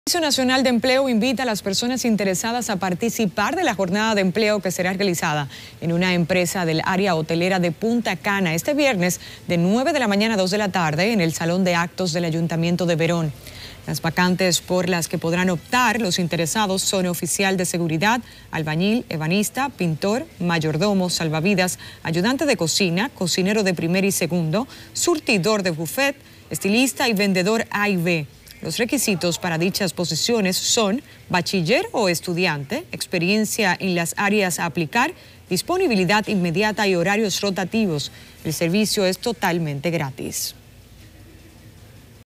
Anuncian jornada de empleo este viernes en Punta Cana
El Servicio Nacional de Empleo invita a las personas interesadas a participar de la jornada de empleo que será realizada en una empresa del área hotelera de Punta Cana, este viernes de 9: 00 a.m. a 2: 00 p.m., en el Salón de Actos del Ayuntamiento de Verón.